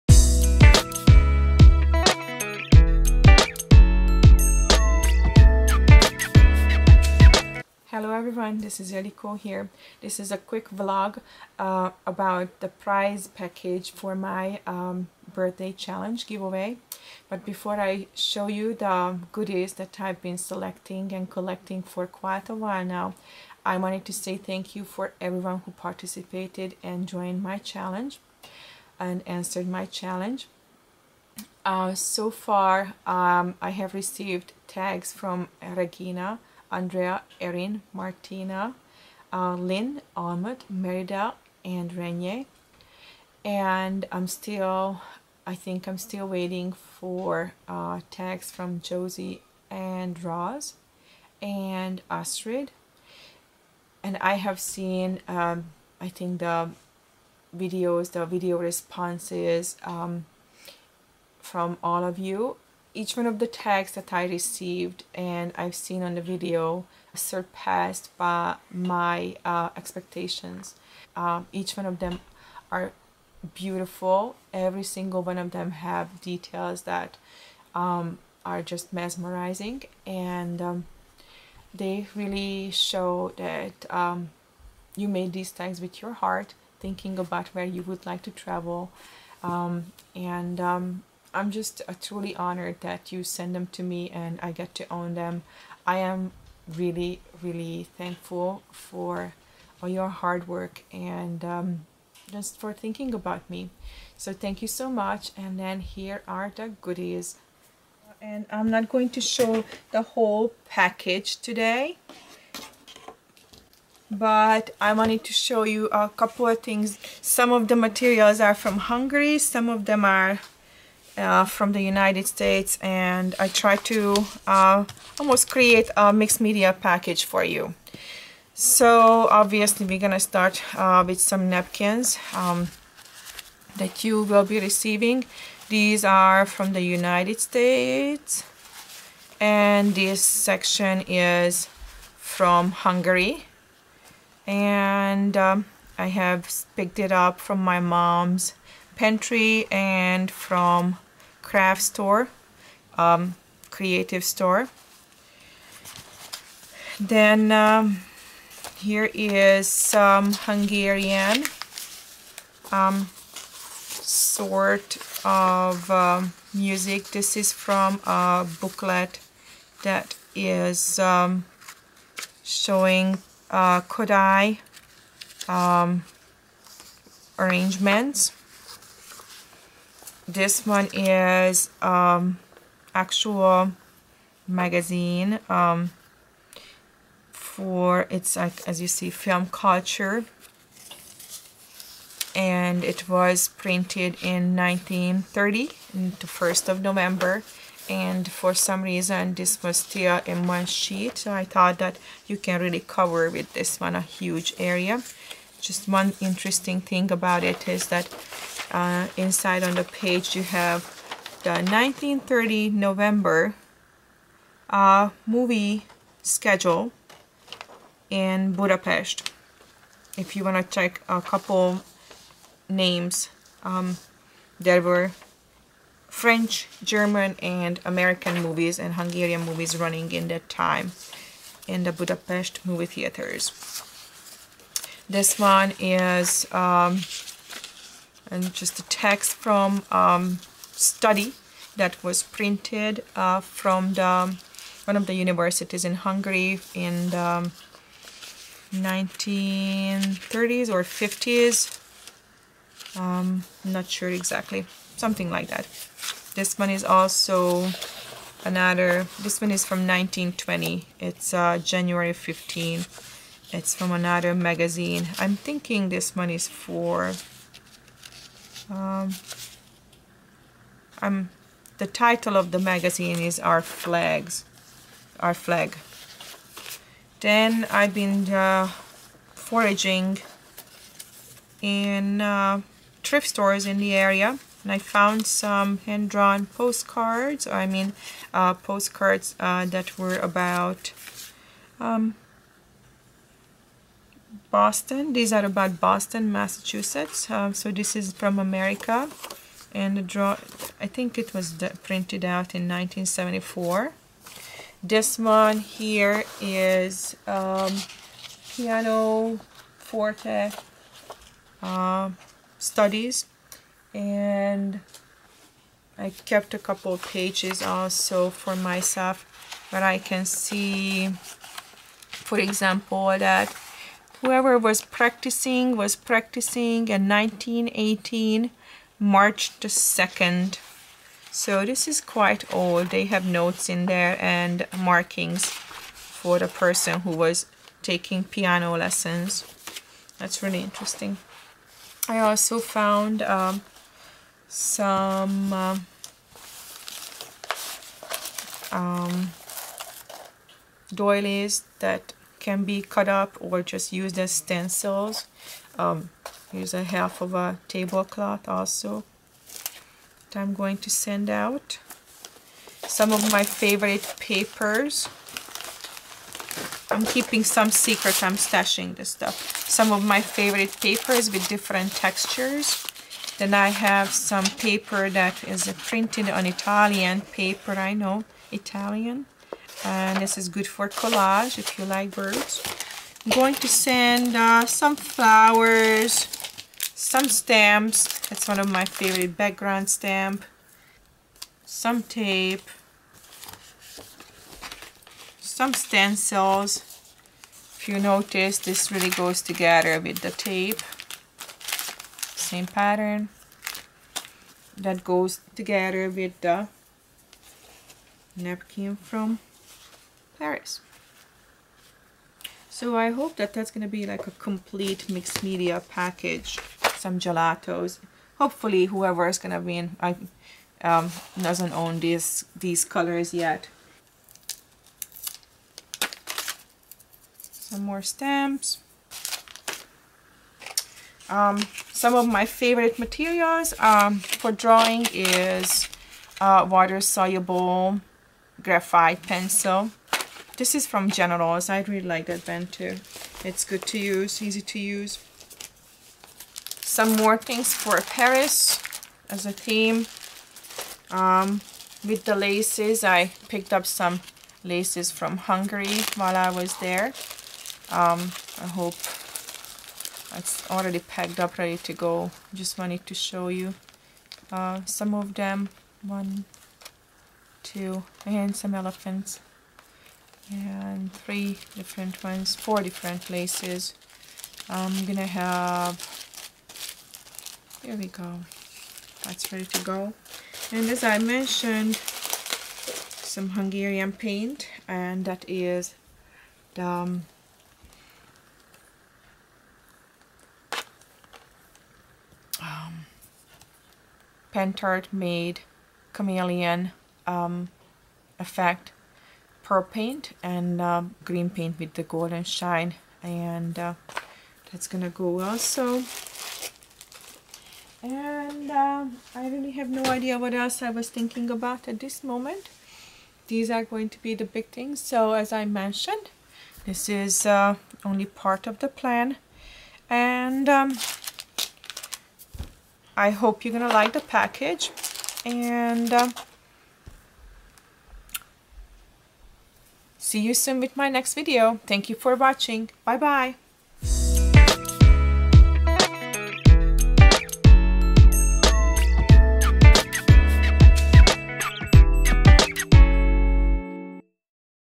Vlog - Prize Package for my Travel Tag Challenge Giveaway
Hello,
Finally, here is the video about the goodies I selected as part of the prize package for my challenge.
I hope you like them. Let me know if you think something is missing.
Ildiko